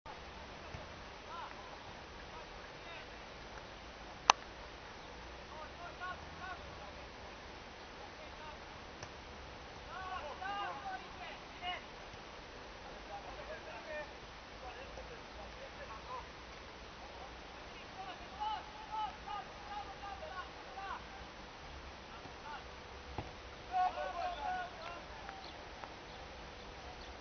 Gol Alexa 2
Golul 2 al lui Alexa la antrenamentul de azi al lui Dinamo